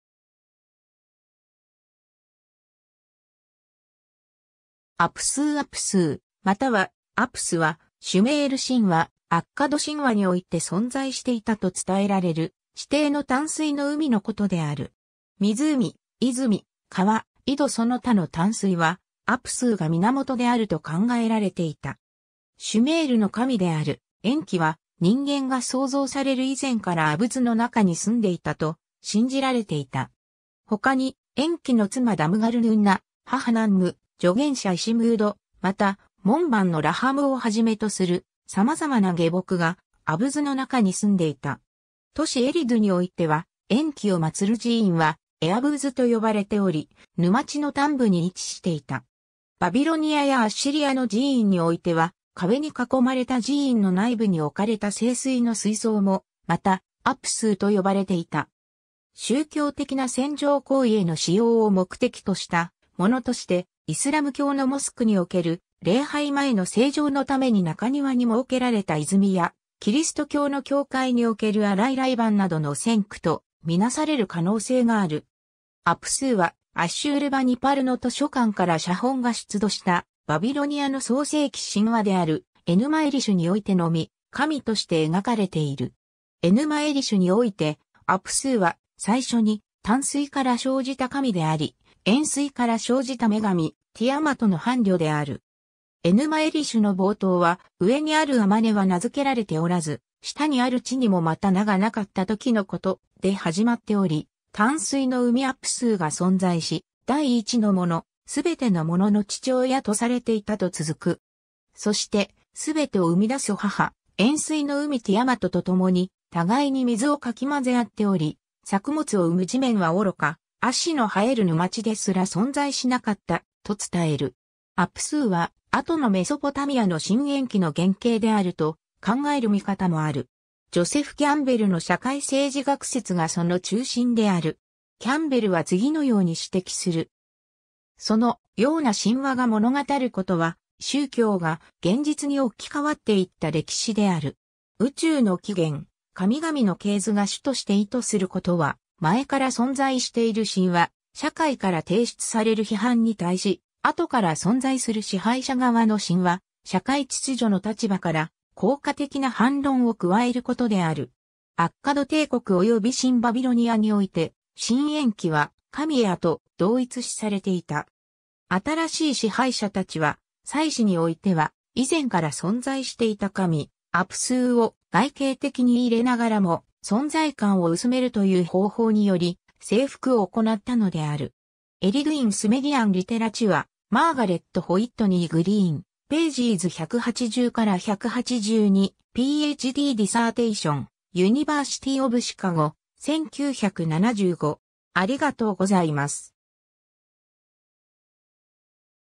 アプスー
エヌマ・エリシュ
アプスー アプスーまたはアプス（apsû、abzu）は、シュメール神話・アッカド神話において存在していたと伝えられる、地底の淡水の海のことである。
湖、泉、川、井戸その他の淡水は、アプスーが源であると考えられていた。
 シュメールの神であるエンキ（アッカド語ではエア）は、人間が創造される以前からアブズ（アッカド語ではアプスー）の中に住んでいたと信じられていた。
他に、エンキの妻ダムガルヌンナ、母ナンム、助言者イシムード、また門番のラハムをはじめとする様々な下僕が、アブズ（アプスー）の中に住んでいた。
 都市エリドゥにおいては、エンキを祀る寺院はエアブズ（E-abzu、アブズの寺院の意）と呼ばれており、沼地の端部に位置していた。
 バビロニアやアッシリアの寺院においては、壁に囲まれた寺院の内部に置かれた聖水の水槽もまた「アプスー」と呼ばれていた。
宗教的な洗浄行為への使用を目的としたものとして、イスラム教のモスクにおける、礼拝前の清浄（ウドゥー）のために中庭に設けられた泉や、キリスト教の教会における洗礼盤などの先駆とみなされる可能性がある。
 アプスーは、アッシュールバニパルの図書館から写本が出土した、バビロニアの創世記神話である『エヌマ・エリシュ』においてのみ、神として描かれている。
『エヌマ・エリシュ』において、アプスーは、最初に淡水から生じた神であり、塩水から生じた女神ティアマトの伴侶である。
『エヌマ・エリシュ』の冒頭は、「上にある天は名づけられておらず、下にある地にもまた名がなかった時のこと…」で始まっており、淡水の海アプスーが存在し、「第一の者、すべてのものの父親」とされていたと続く。
そして、すべてを生み出す母、塩水の海ティアマトとともに、互いに水をかき混ぜあっており、作物を生む地面はおろか、葦の生える沼地ですら存在しなかった、と伝える。
 アプスーは、後のメソポタミアの神エンキ（エア）の原型であると考える見方もある。
ジョセフ・キャンベル（Joseph Campbell）の社会政治学説がその中心である。
キャンベルは次のように指摘する。
「…そのような神話が物語ることは、（その土地・文明の）宗教が現実に置き換わっていった歴史である。」「宇宙の起源・神々の系図が主として意図することは、前から存在している神話・社会から提出される批判に対し、後から存在する支配者側の神話・社会秩序の立場から、効果的な反論を加えることである。」アッカド帝国および新バビロニアにおいて、神エンキは、神エアと同一視されていた。
新しい支配者たちは、祭祀においては以前から存在していた神アプスーを外形的にいれながらも、存在感を薄めるという方法により「征服」を行ったのである。
 ^ Eridu in Sumerian Literature, Margaret Whitney Green, pages 180-182, Ph.D. dissertation, University of Chicago, 1975.